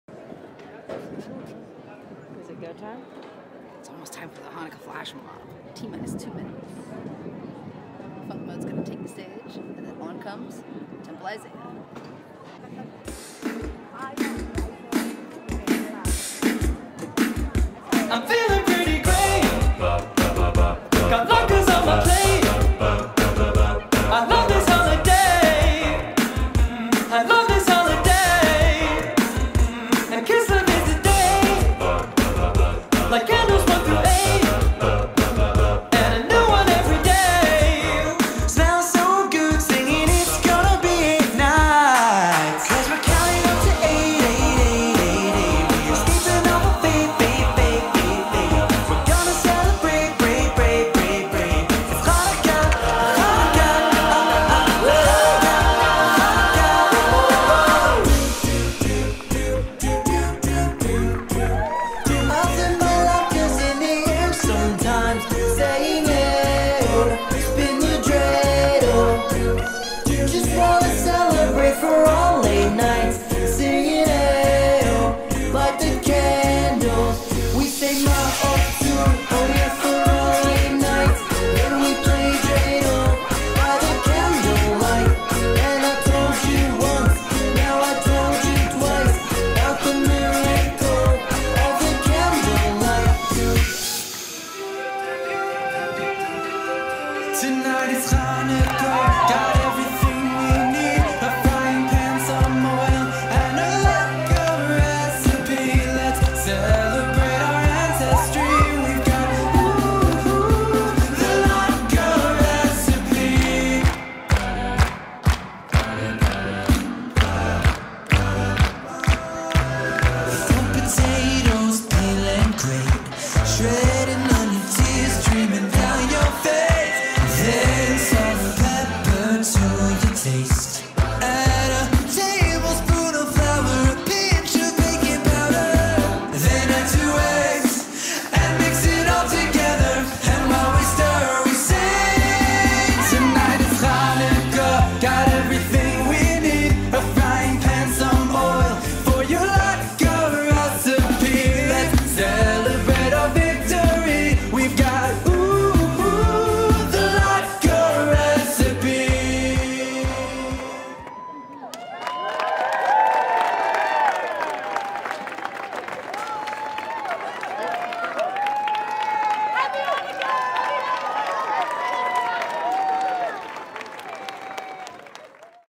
Chanukah Flash Mob - Temple Isaiah with FUNKMODE - 12/17/2017
Temple Isaiah and Rabbi Nicki Greninger recruited FUNKMODE to help them celebrate the Chanukah season with a flash mob at Broadway Plaza in downtown Walnut Creek.

All love,

- FUNKMODE

Classes are now in session ... We're waiting for YOU to come be in the next video!